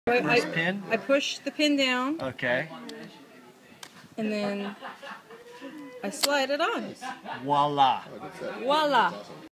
Mounting Transmitter
If you are having trouble mounting your transmitter onto camera hotshoe. Especially older Nikon cameras.